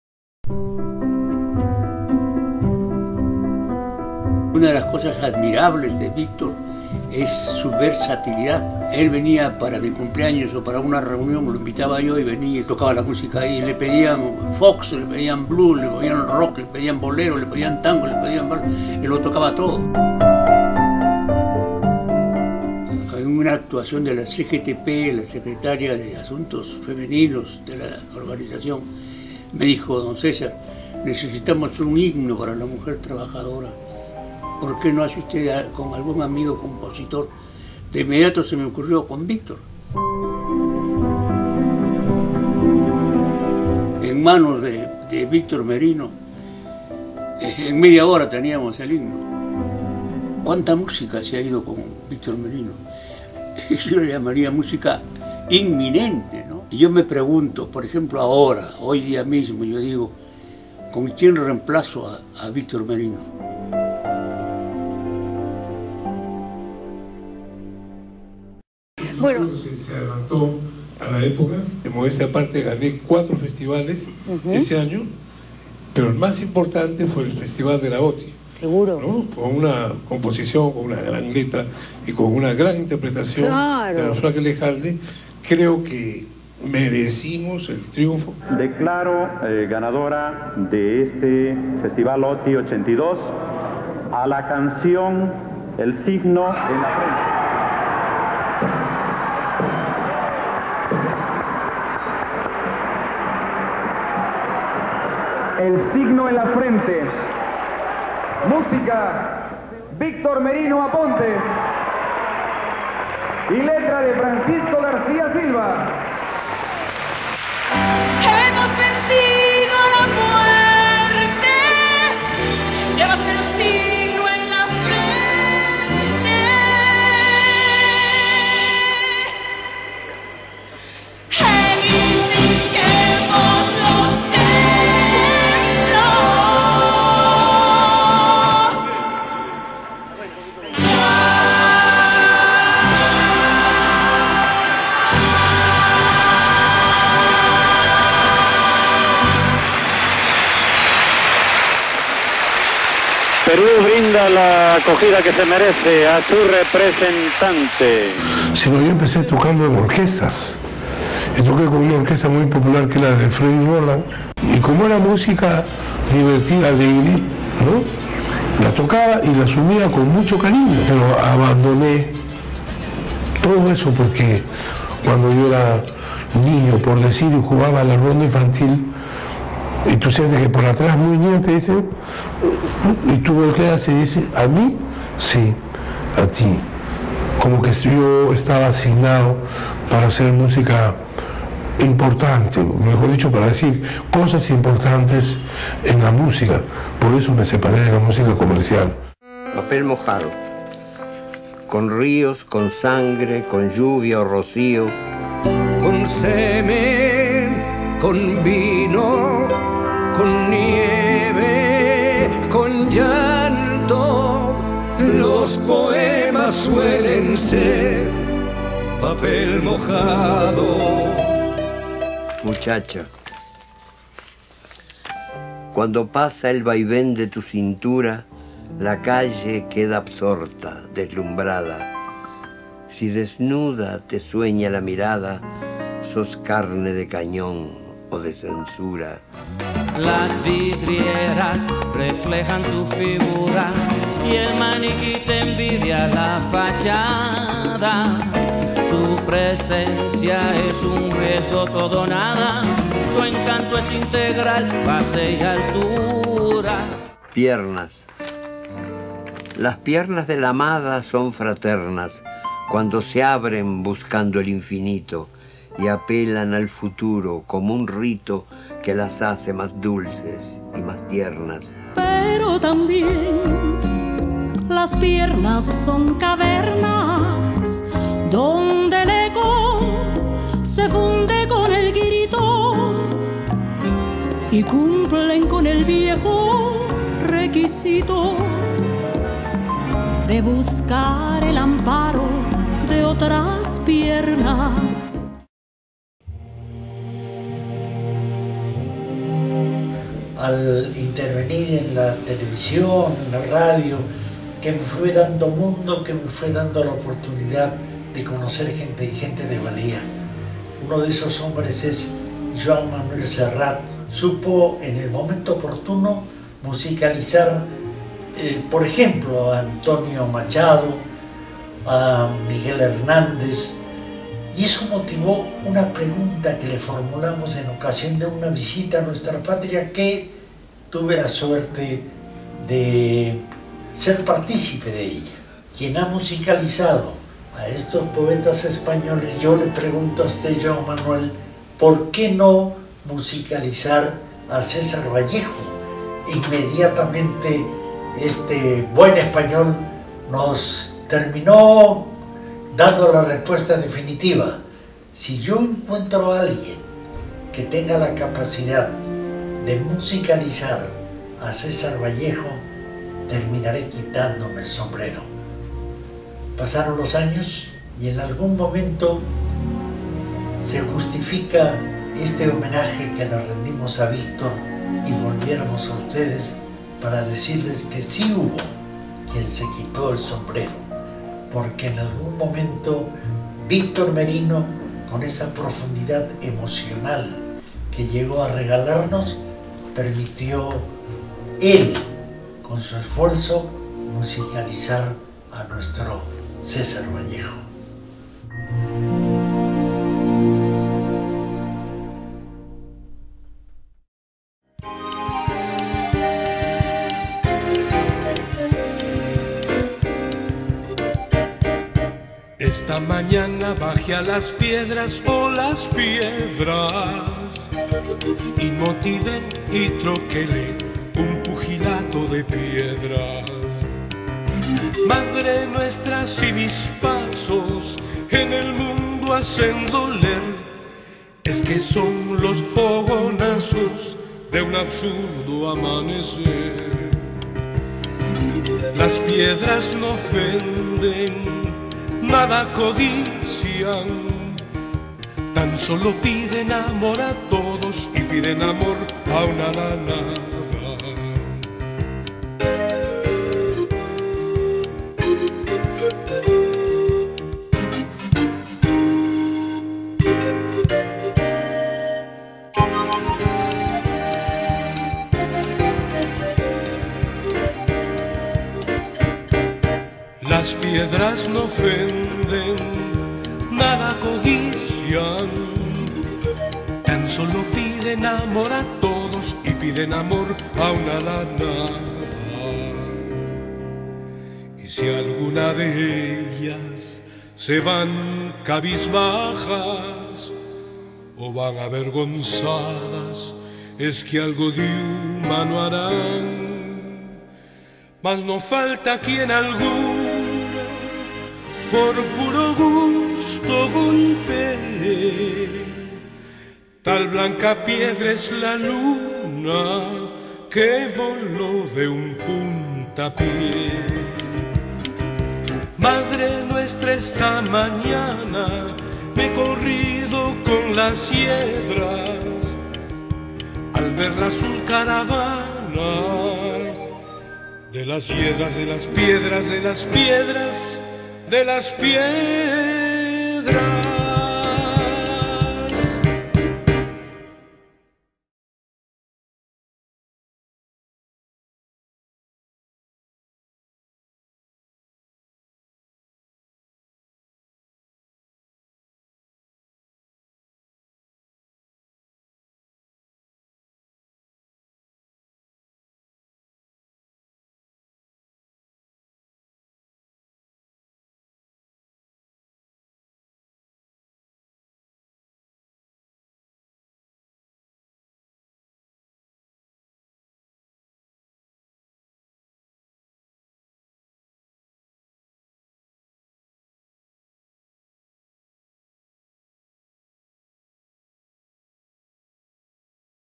VICTOR MERINO - Elegido por la música - Parte 2
Vídeo semblanza que repasa los hitos musicales en la fructífera y extensa trayectoria del gran músico, pianista y compositor Víctor Merino. Apoyado con testimonios de personalidades que conocieron de cerca su enorme sensibilidad y capacidad creadora.

GUIÓN, PRODUCCIÓN Y DIRECCIÓN GENERAL
Paco García y Anthony Ruiz

EDICIÓN Y POST PRODUCCIÓN
Ricardo Ramírez
Reccrea Films